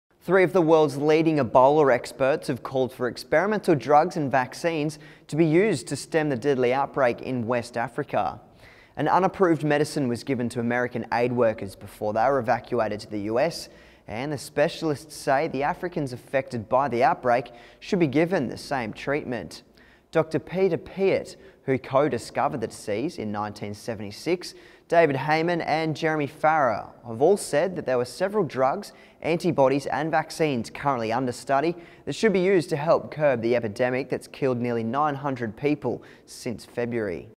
Push To Use Experimental Drugs Against Ebola - August 7, 2014
Three of the world’s leading Ebola experts have called for experimental drugs and vaccines to be used to stem the deadly outbreak in West Africa